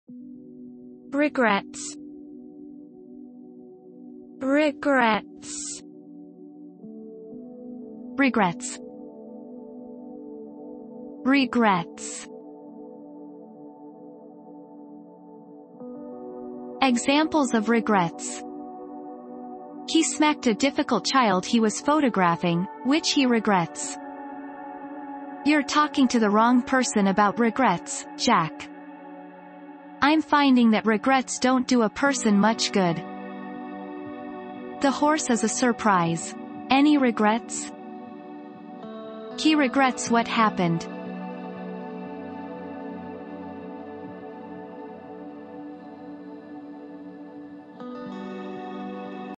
REGRETS pronunciation • How to pronounce REGRETS
How to say Regrets in British English and American English?